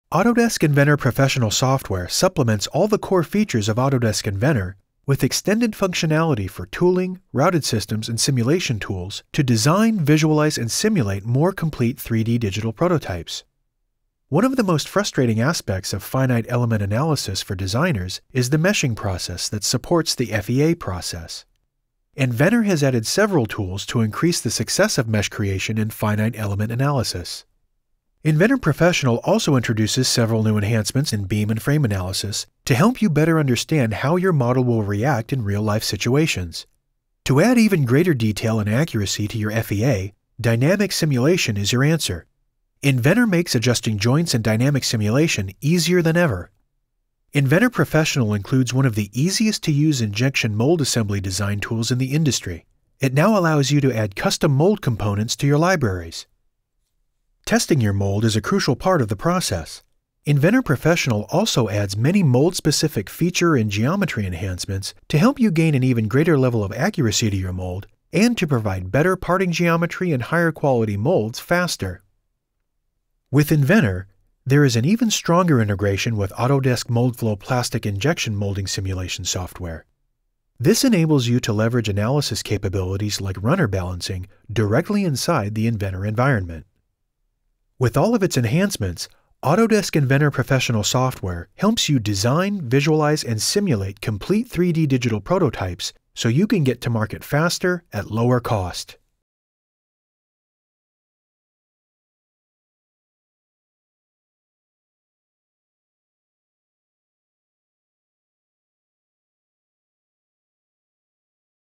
Autodesk Inventor Professional 2012 Enhancements
Autodesk® Inventor® Professional software offers all the core features of Autodesk Inventor 2012, plus extended tooling, routed systems, and simulation tools to design, visualize, and simulate complete 3D digital prototypes.

Simulation Enhancements:  Inventor Professional 2012 introduces several tools that increase the success of mesh creation and adds ease-of-use improvements to the simulation environment.

Thin wall meshing for sheet metal and plastic parts

Improved beam analysis

Easily adjust joints in dynamic simulation

Tooling Enhancements:  Inventor Professional 2012, one of the easiest-to-use injection mold assembly design tools, now allows you to add custom mold components to your libraries.

Other enhancements include:

Mold-specific feature and geometry enhancements

User-defined mold bases and mold components

Improved integration with Autodesk® Moldflow® plastics injection molding simulation software